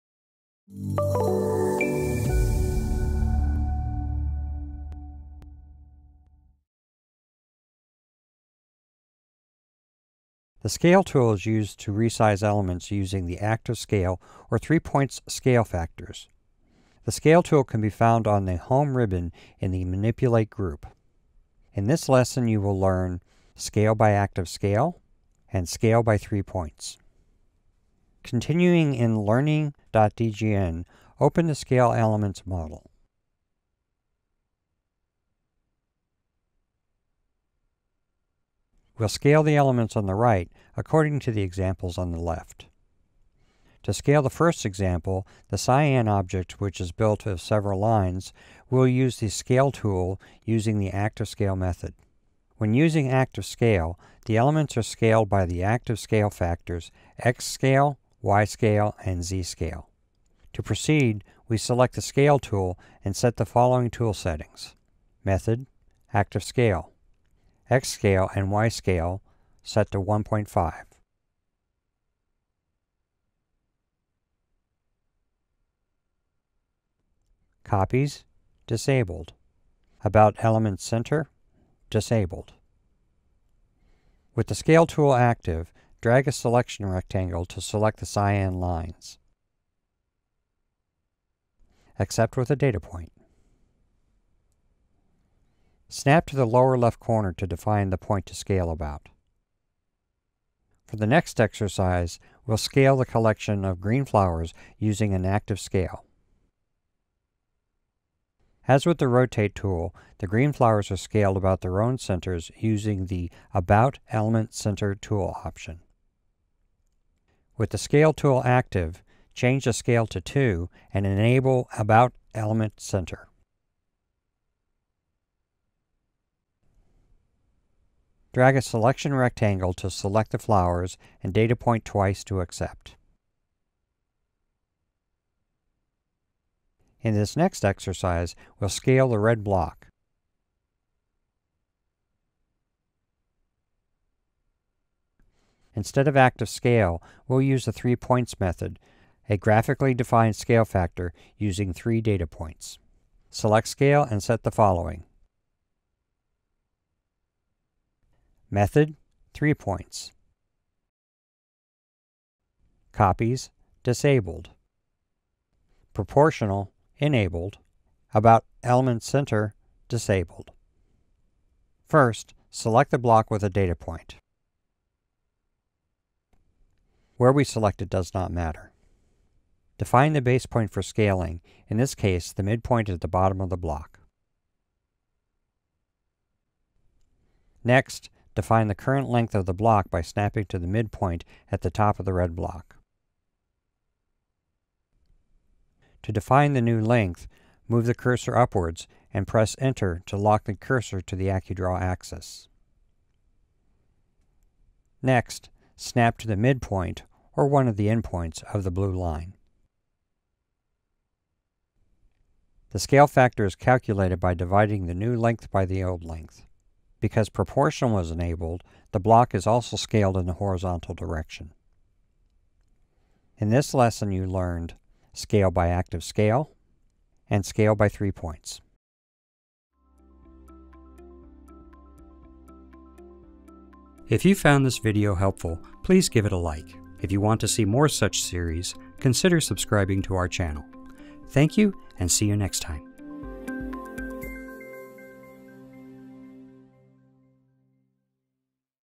5 Scale
Elements often need to have their size adjusted. This can be done via the Scale command. In this video you will learn to use the Scale tool in a variety of ways, via the elements center and using a scale factor or interactively.